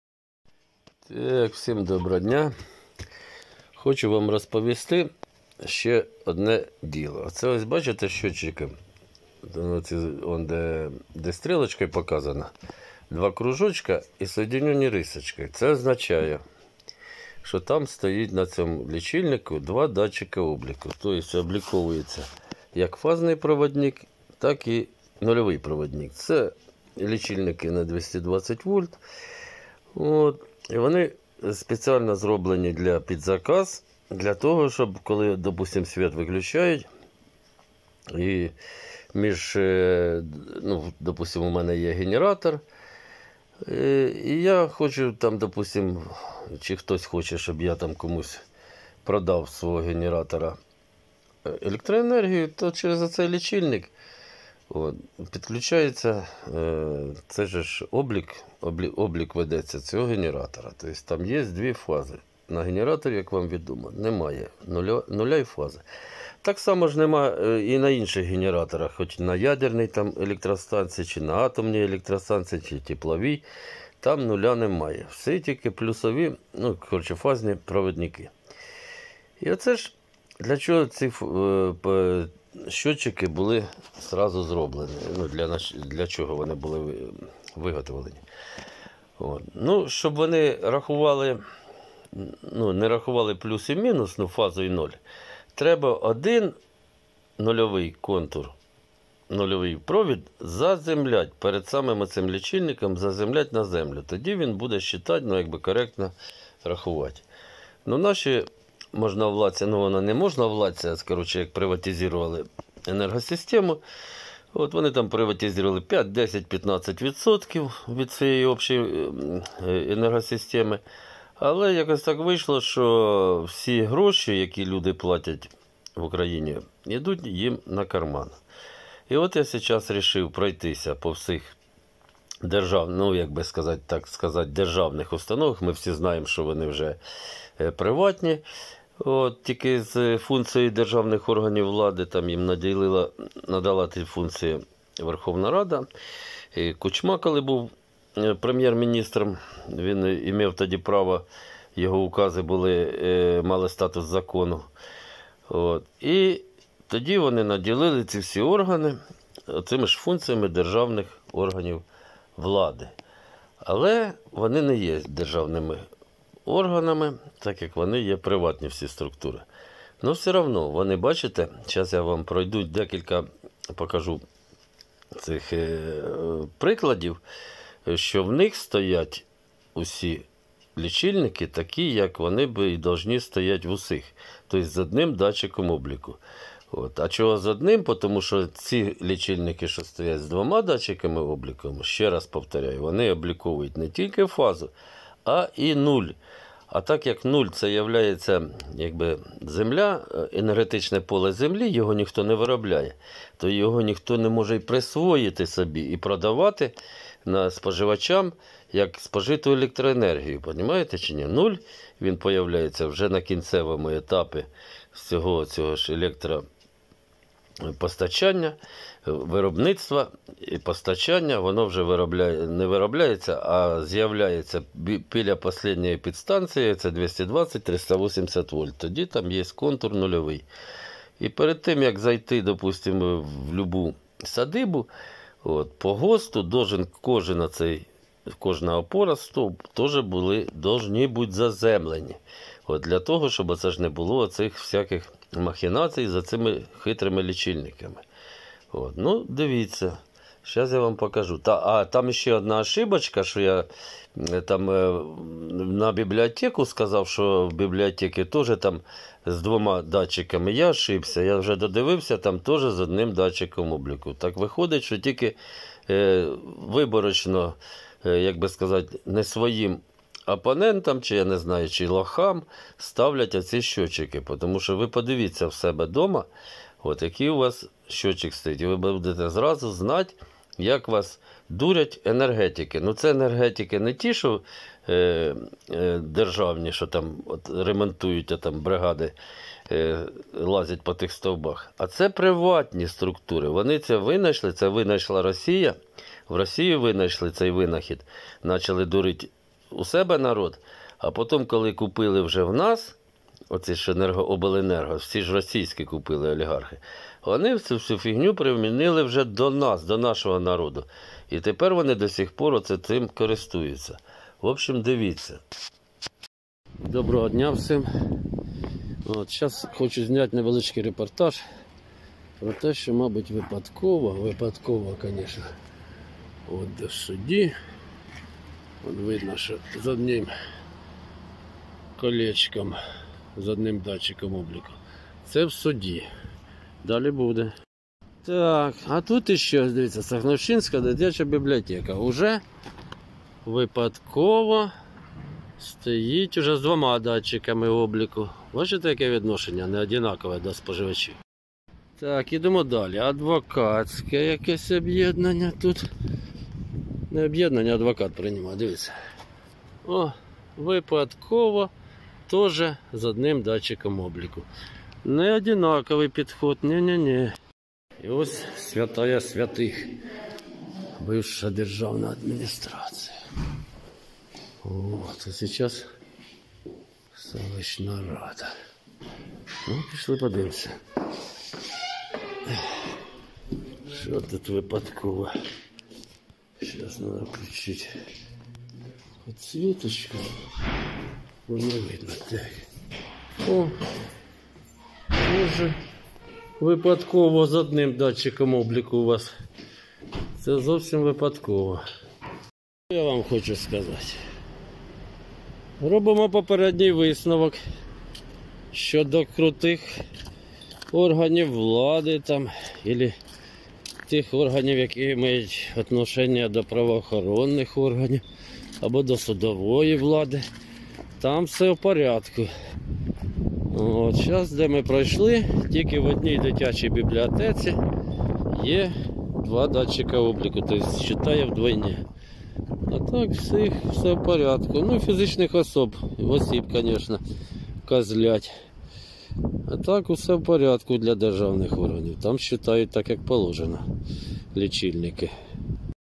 ЯК НАС ДУРЯТЬ НА ЛІЧИЛЬНИКАХ ЕНЕРГЕТИКИ
Придумано в Росії, застосовано олігархам в Україні